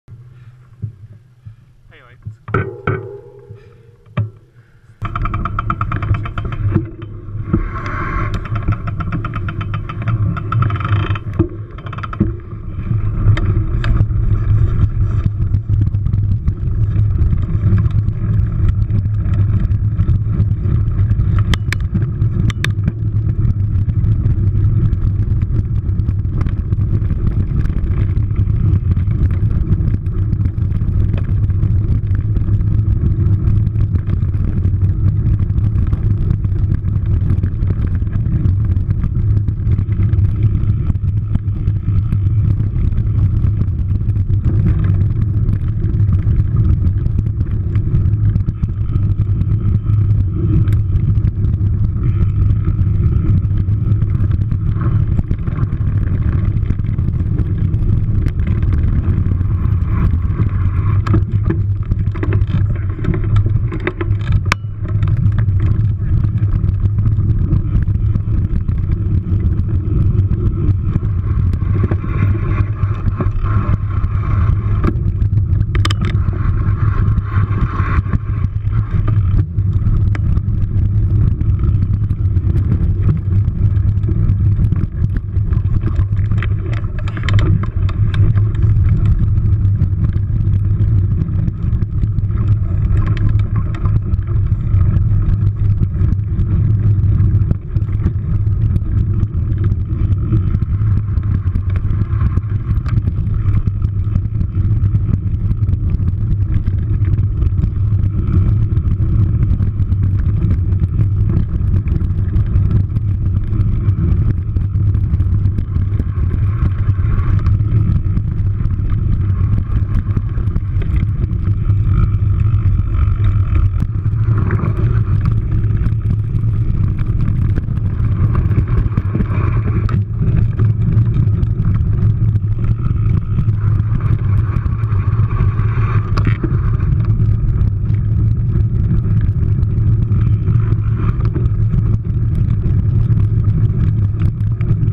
Riding with Katz
Mountain biking at Gamehaven and Eastwood with Katz. Great times!  Katz rode the Trek Farley and I was on the Superfly 100.